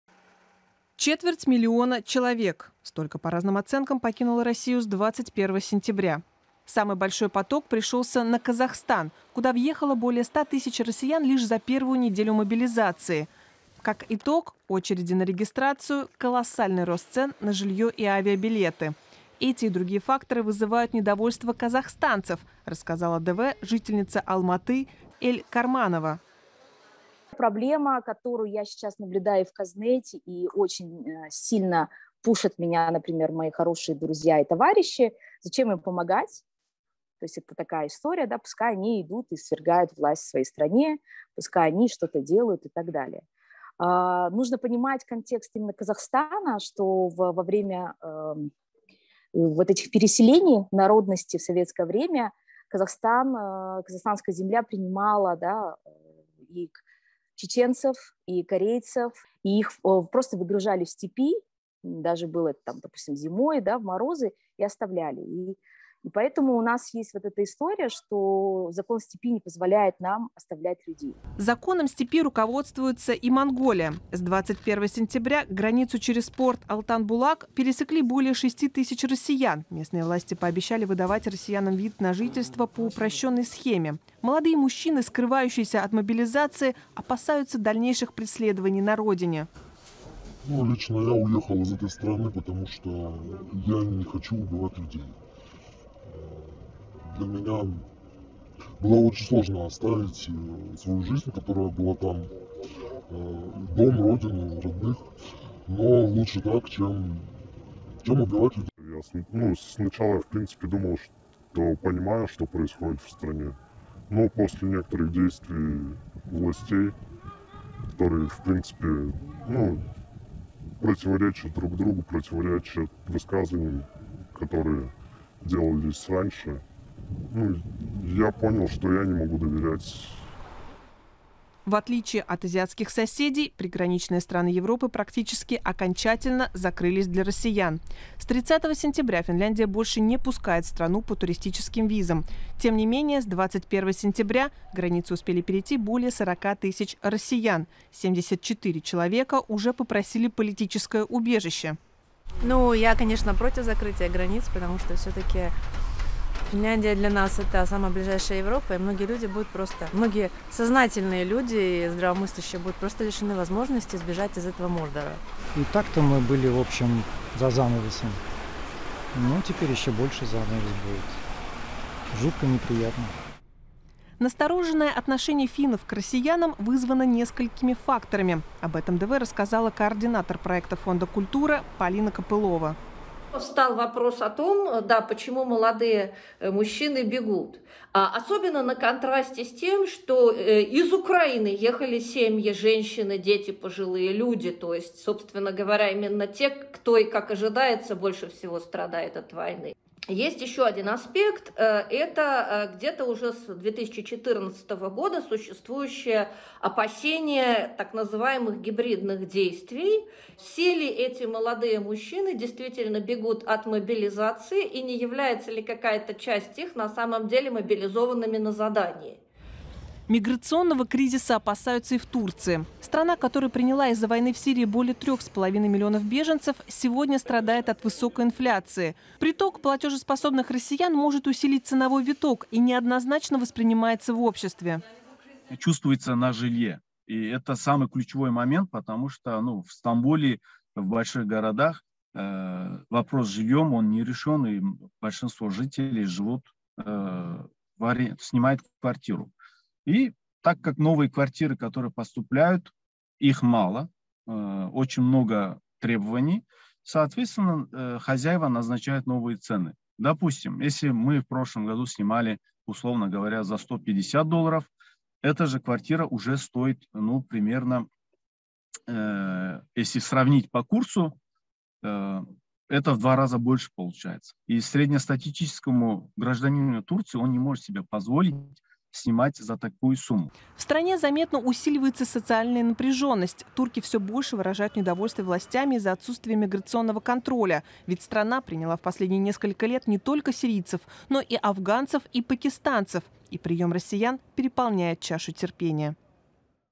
Как бегущих от мобилизации россиян принимают в Монголии, Казахстане, Турции и ЕС
По разным оценкам Россию с 21 сентября покинуло более четверти миллиона человек. Что их ждет заграницей?  И почему Монголия и Казахстан принимают бегущих от мобилизации россиян, а ЕС закрывает границы?